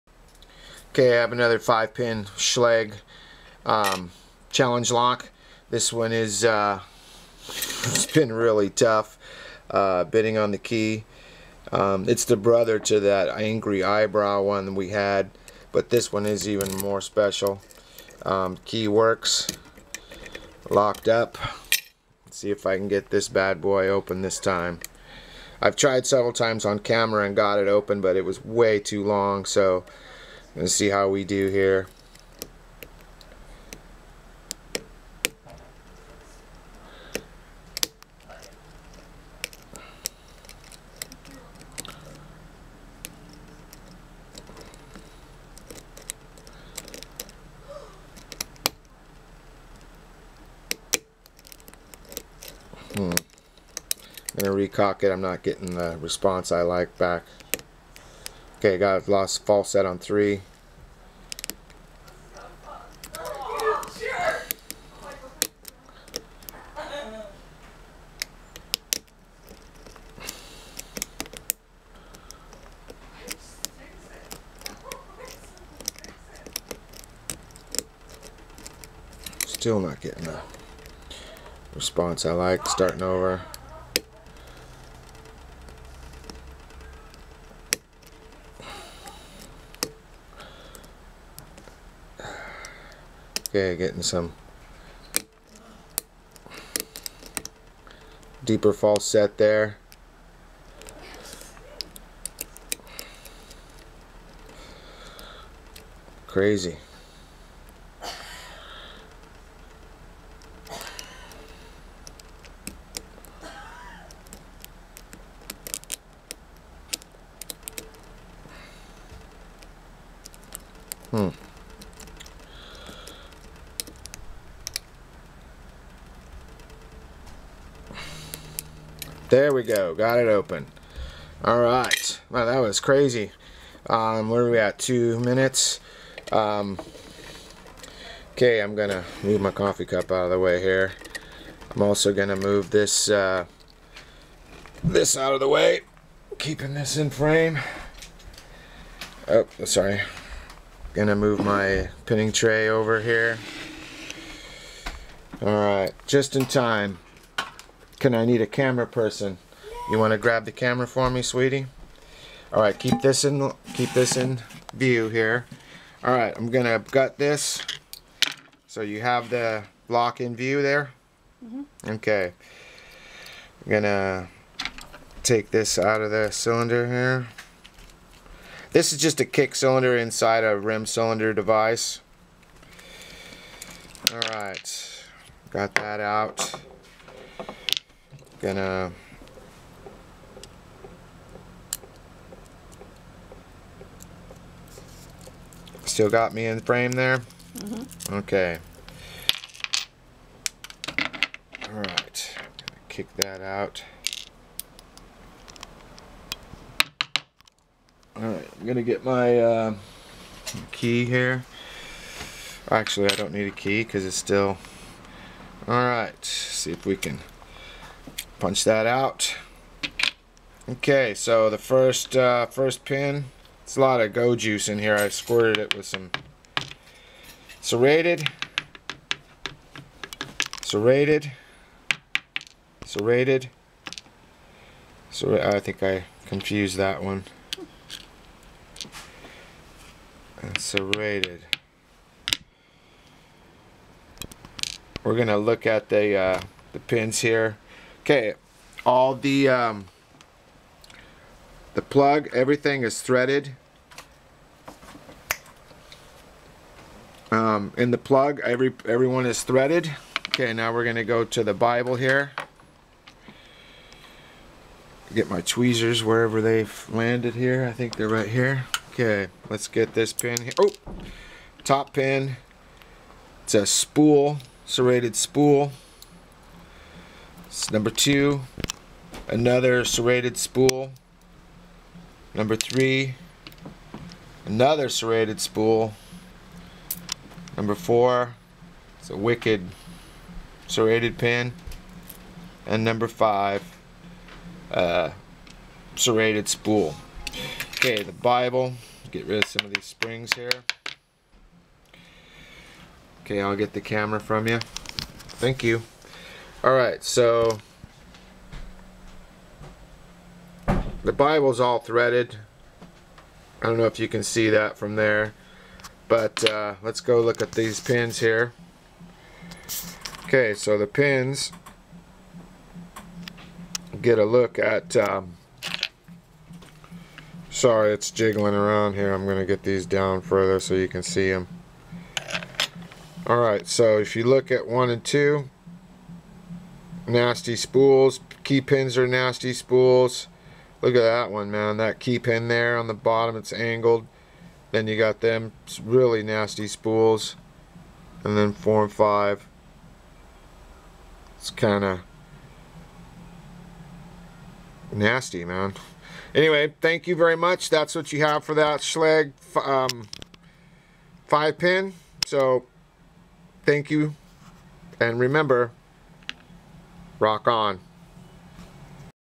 Wicked 5-pin Schlage.Threaded everything, tough pick
This is the brother lock to the previous lock I picked on camera. It is way more tough than I thought. The lock has a very precise picking order, If you make one mistake, it's start over time.